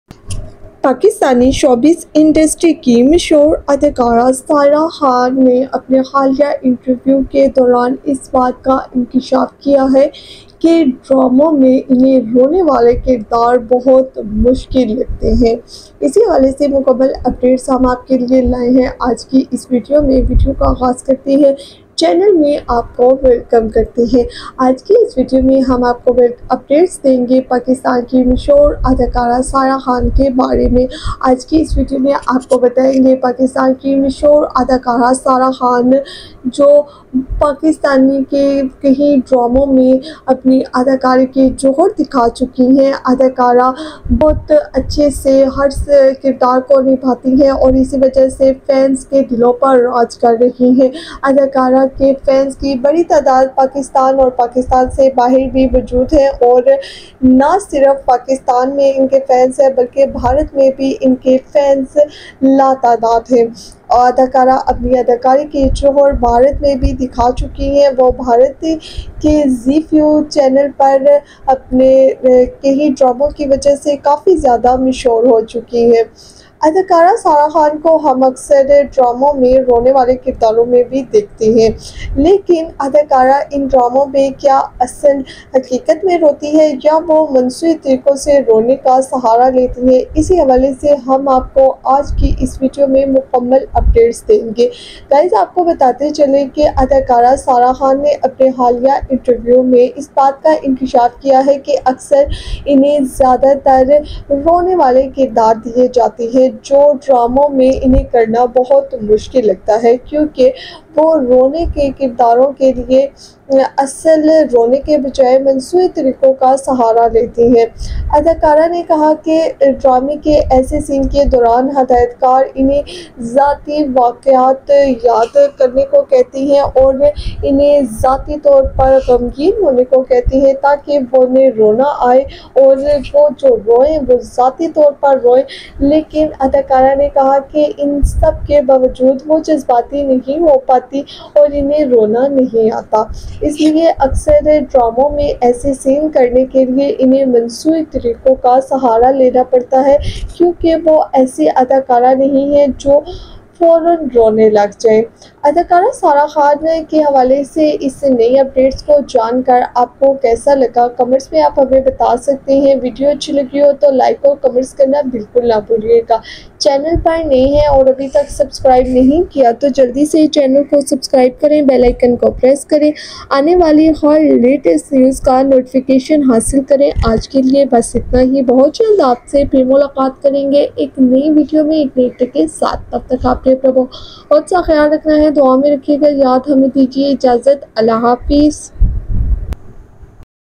I use artificial methods to cry in plays, Sarah Khan
I Use Artificial Methods to Cry in Plays | Sarah Khan Interview | Acting Techniques

Description:

In this exclusive interview, renowned actress Sarah Khan reveals her unique approach to acting, including the artificial methods she uses to cry on demand for emotional scenes in plays and dramas. Learn how Sarah perfects her craft and maintains authenticity, even when tears don’t come naturally.

In this video, Sarah Khan covers:

- How actors use artificial techniques to cry
- The psychology behind delivering emotional performances
- Sarah Khan's journey in the entertainment industry
- Key tips for upcoming actors to improve their emotional acting skills

Don’t miss out on this revealing conversation, where Sarah shares valuable acting advice and her thoughts on the pressure actors face during intense scenes.

Tags.